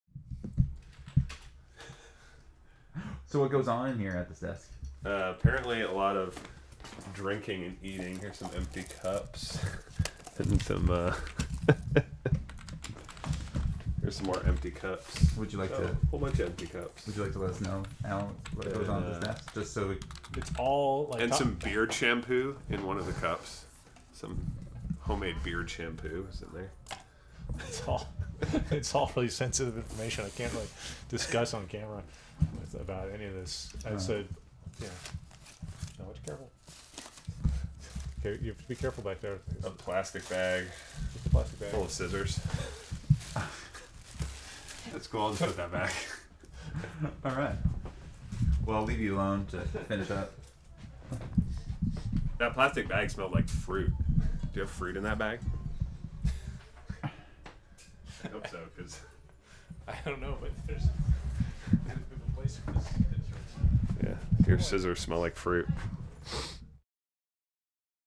It Came from Aquarius Records - Your Scissors Smell like Fruit
The inner workings of Aquarius Records - Andee gives us a tour of Allan's workspace.

Produced, Directed, and Edited by Kenneth Thomas.

Summer 2020.